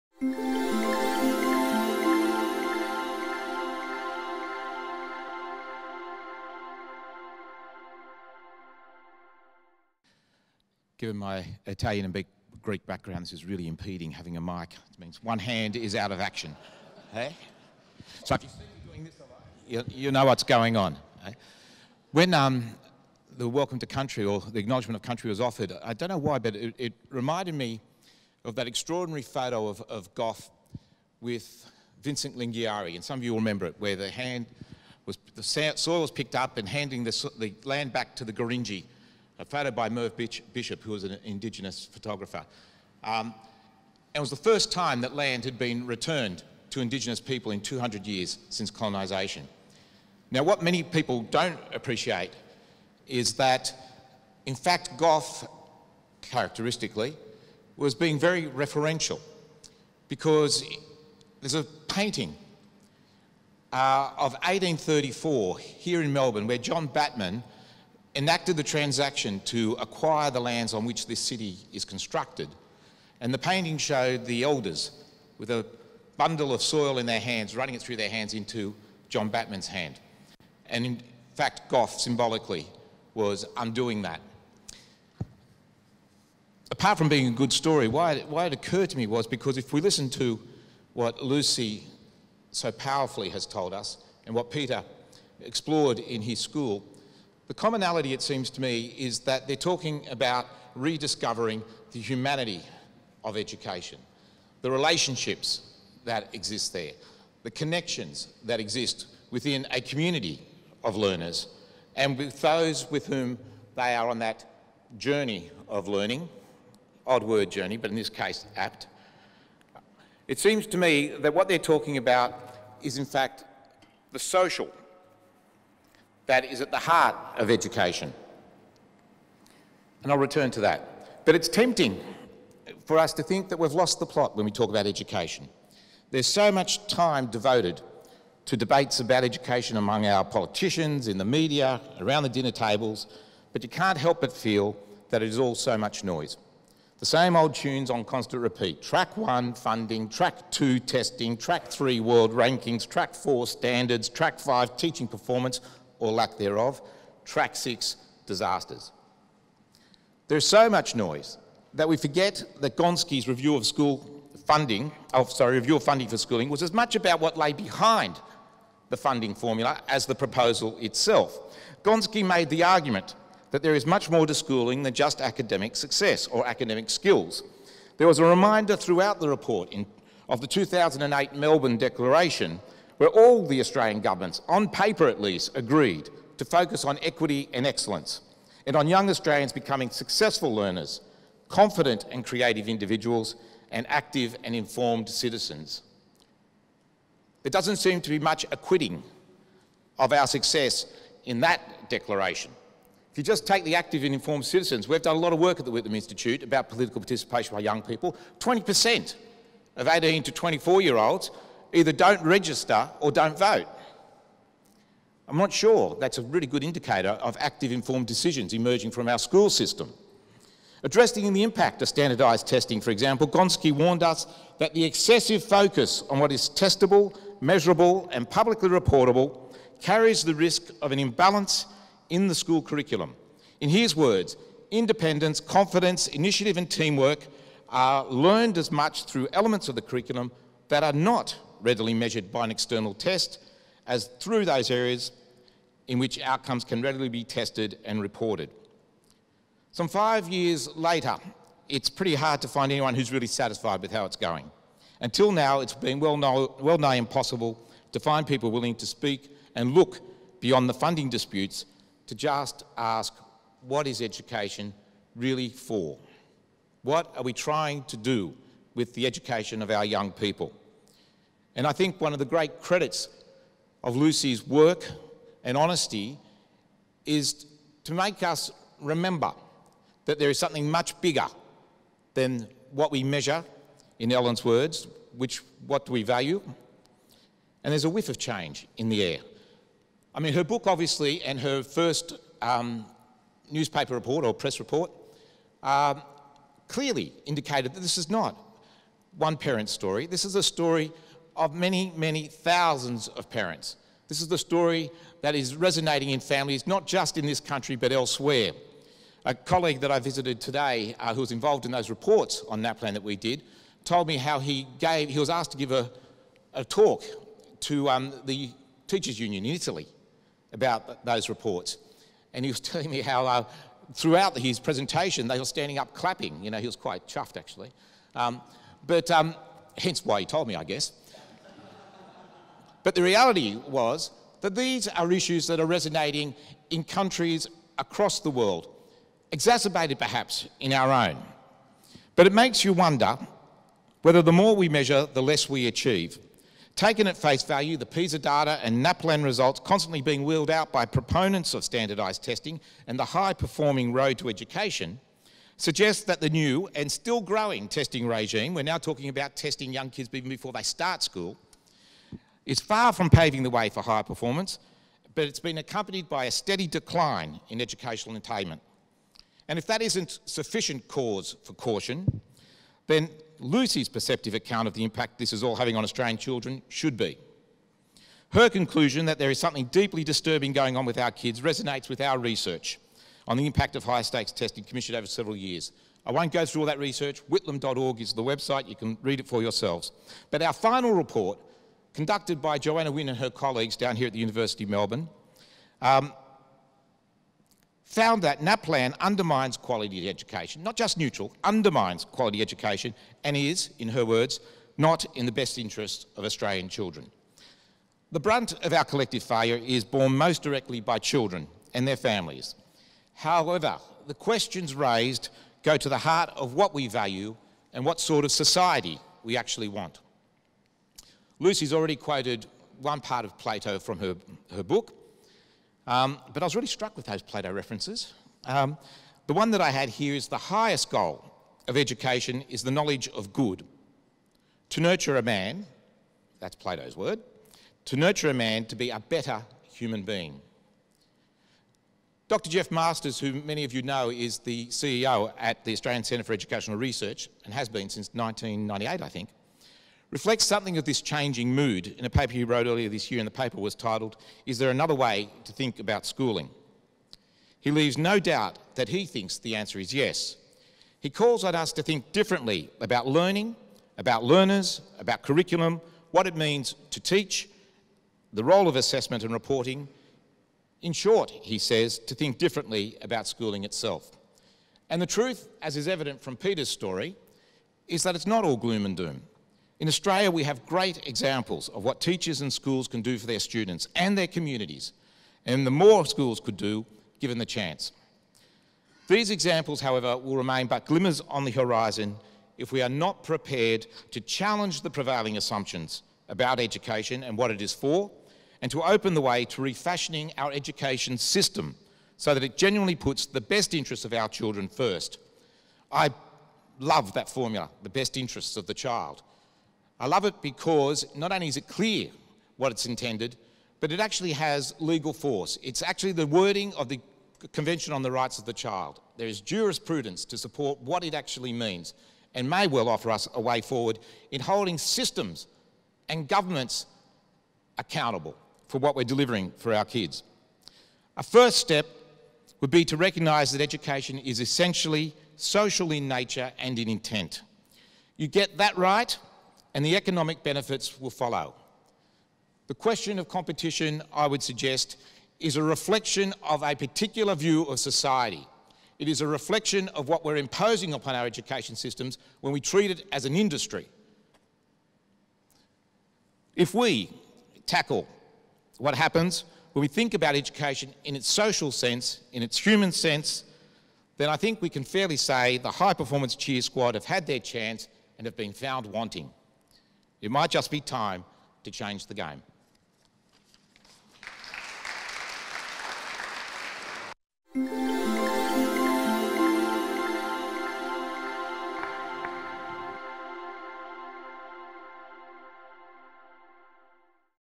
Beautiful Failures - Eric Sidoti presentation
Presentation at the Australian Learning Lecture event 'Lucy Clark: Beautiful Failures' with Lucy Clark, author, Beautiful Failures; Peter Hutton, Principal, Templestowe College, and Eric Sidoti, Director, Whitlam Institute.